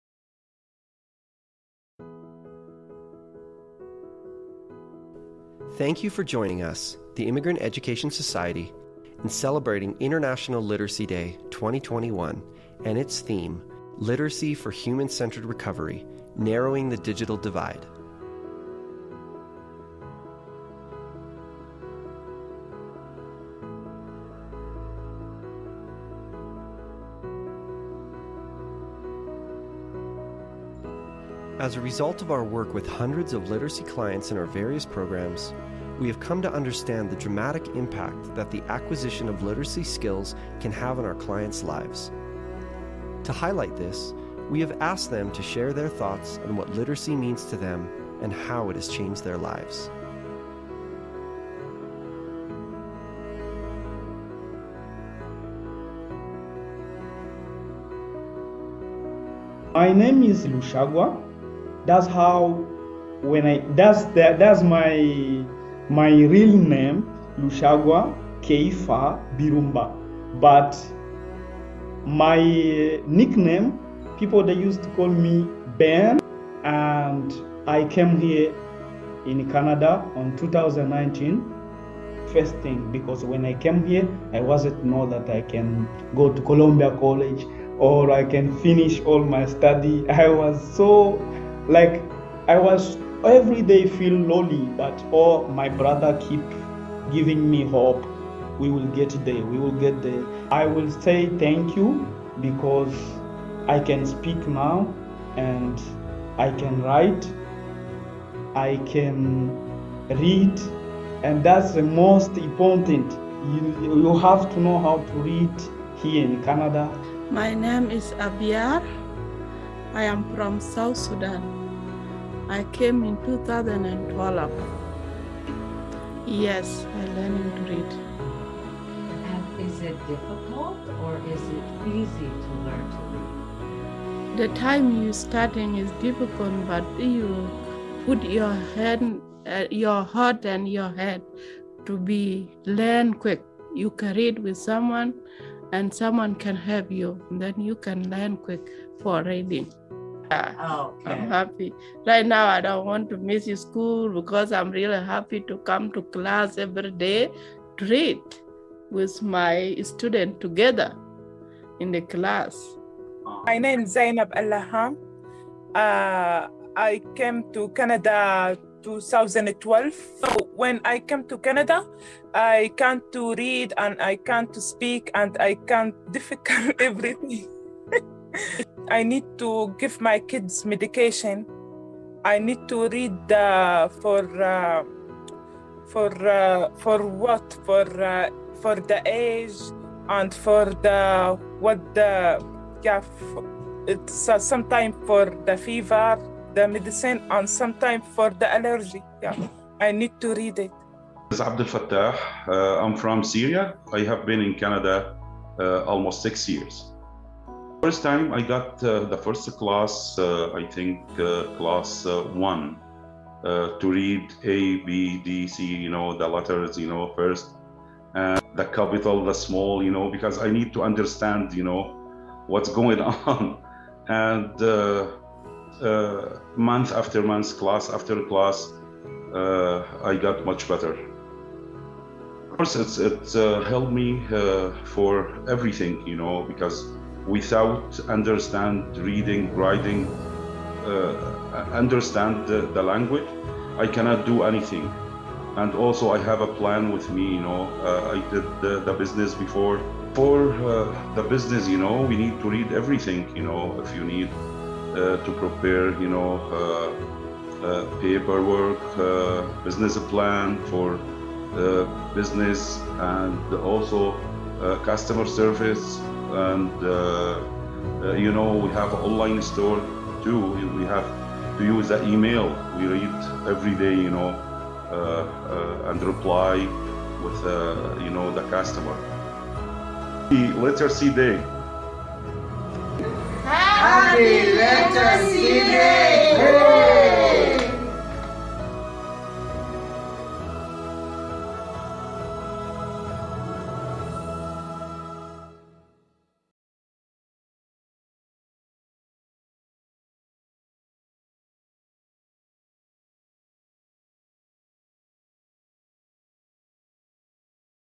International Literacy Day - September 8, 2021
Current and former TIES literacy learners tell us what literacy means to them and why it is an important part of their lives.